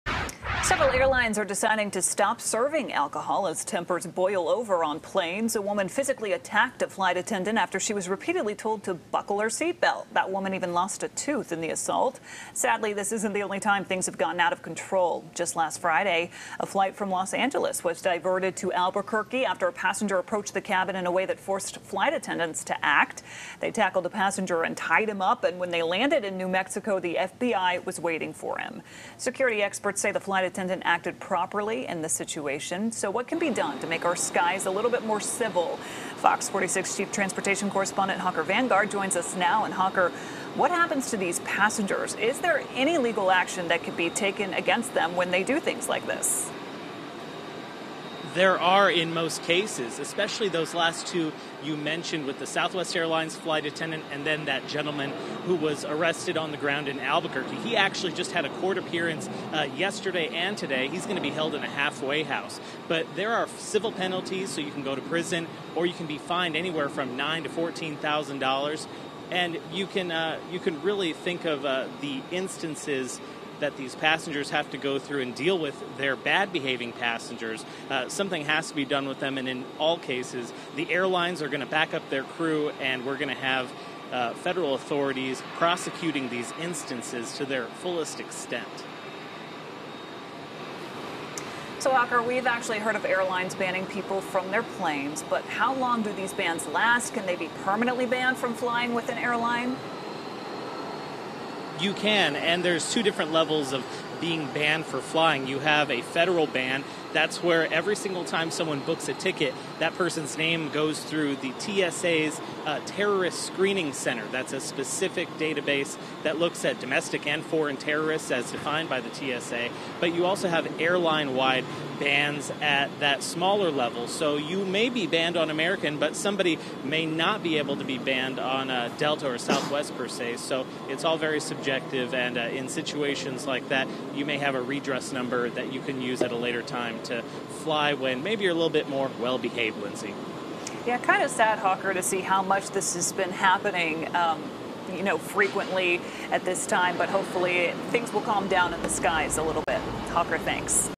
Rise in Airline Assaults: Several airlines stop serving alcohol as tempers boil on planes
A woman physically attacked a flight attendant after she was repeatedly told to buckle her seat belt - that woman even lost a tooth in the assault.